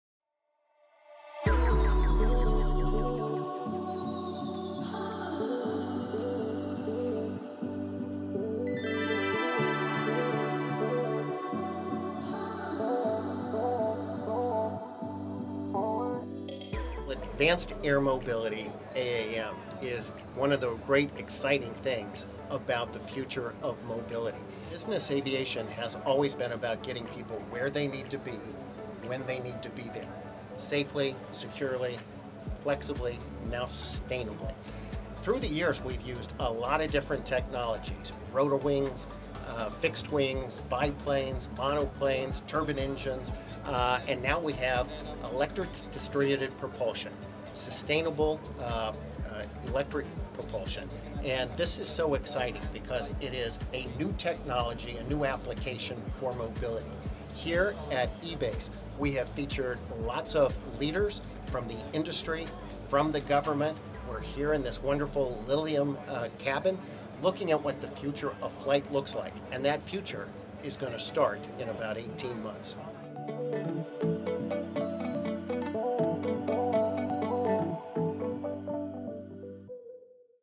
EBACE TV: Inside Lilium’s New Air Taxi Cabin With NBAA’s Ed Bolen
Seated inside the newly unveiled passenger cabin for Lilium’s electric vertical takeoff and landing aircraft at the 2023 European Business Aviation Convention & Exhibition (EBACE2023), NBAA President and CEO Ed Bolen shares insights about the importance of technological innovation in business aviation.